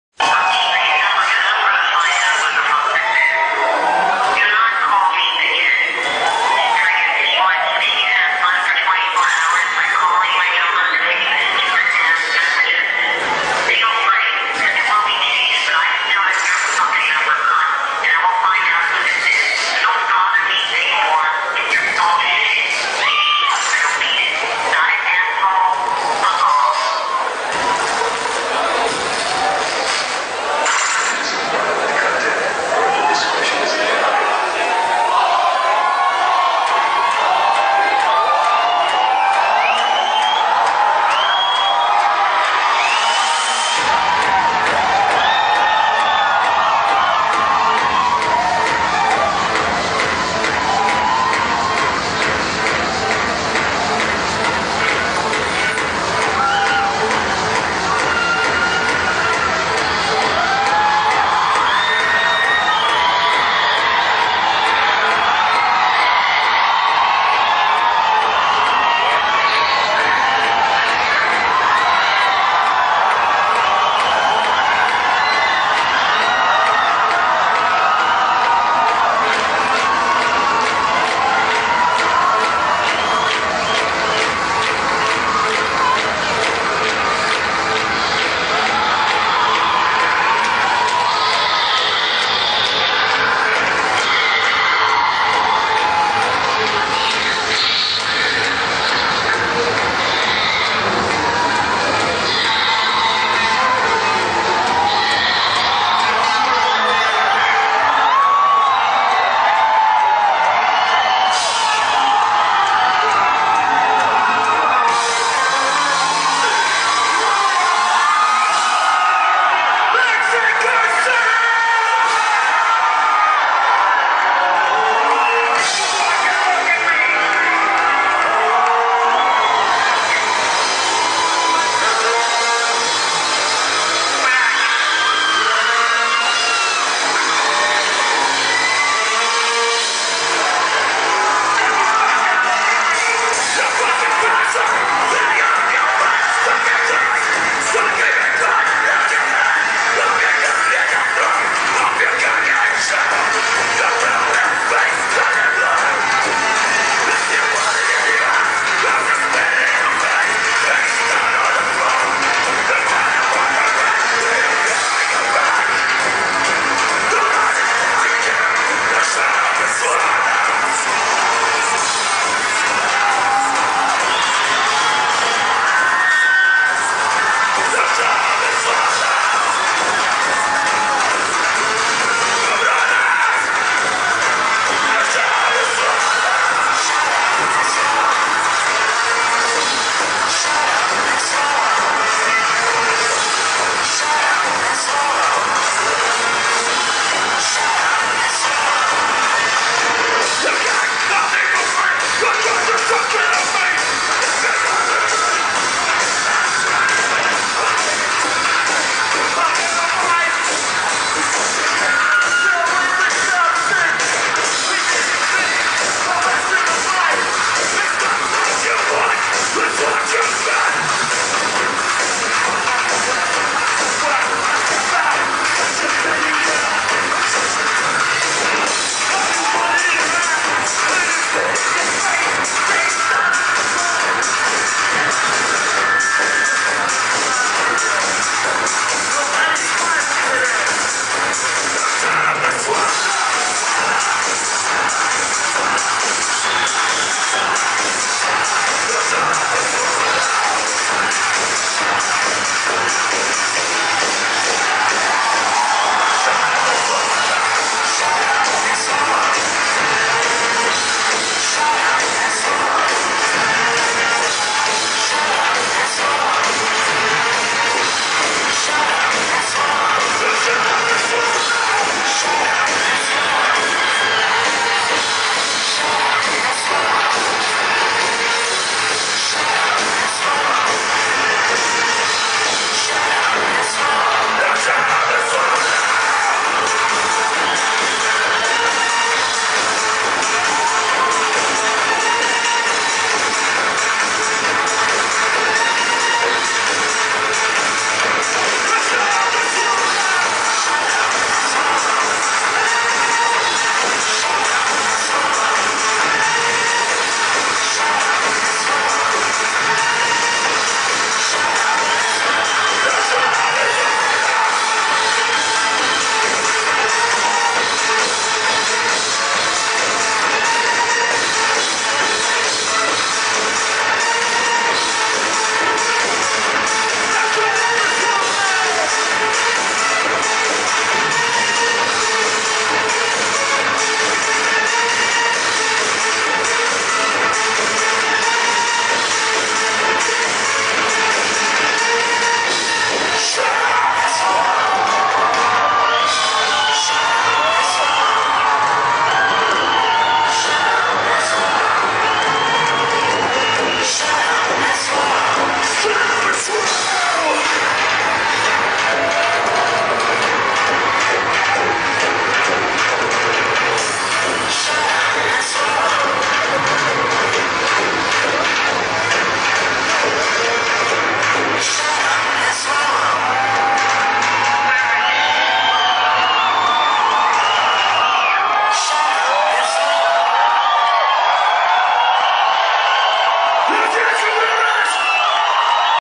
Combichrist Mexico 2008
este fuel el ultimo concert en mex df en tlatelolco y estuvo weno...